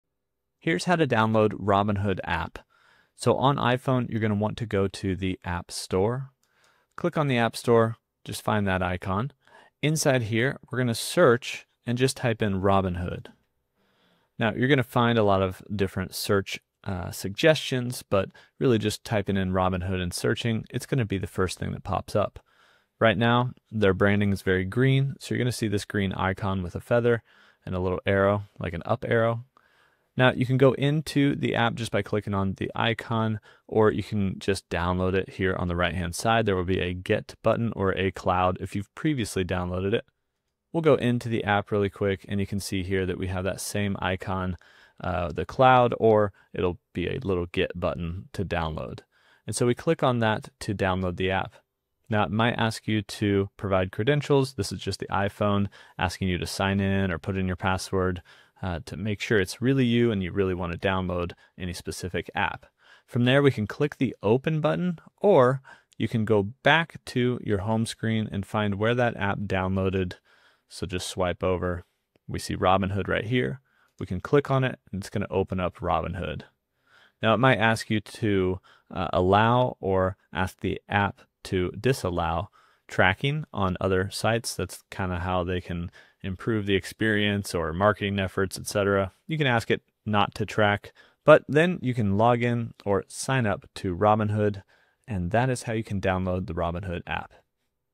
How to Download Robinhood
In this Robinhood tutorial, learn how to download Robinhood on iPhone.

Get a free stock on Robinhood!